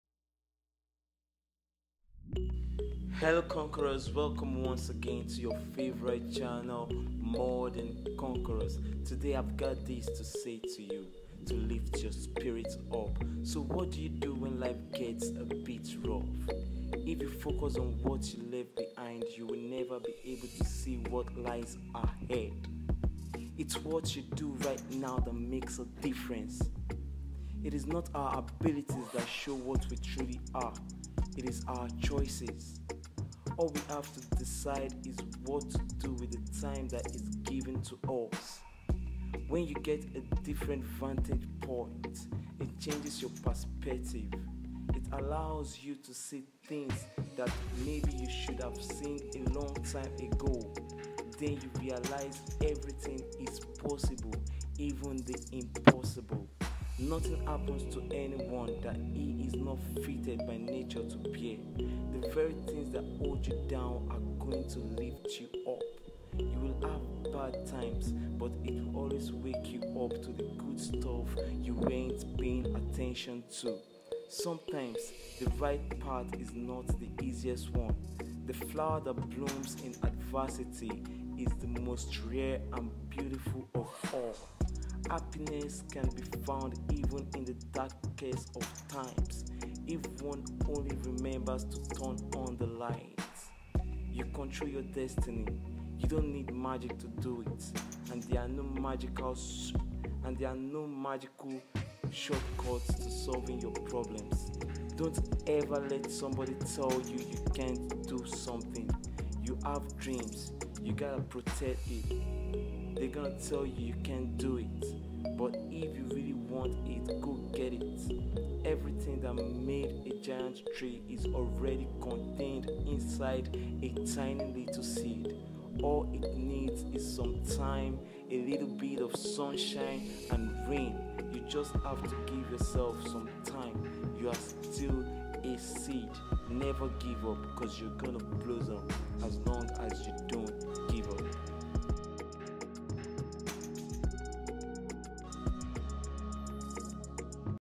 motivational speech for hard times
Here is a motivational speech to keep you going in hard times, times that you feel like giving up listen to this speech and get your spirit ignited